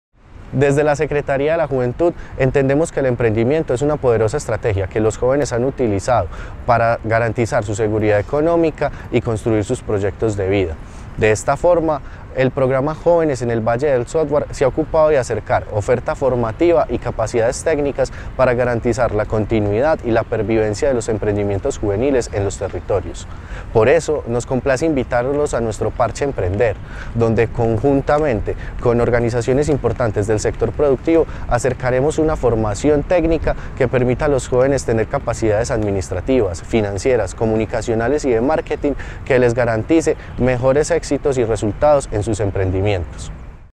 Secretario Santiago Bedoya Parche Emprender
Medellín lanza Parche Emprender una estrategia de formación virtual para 150 jóvenes emprendedores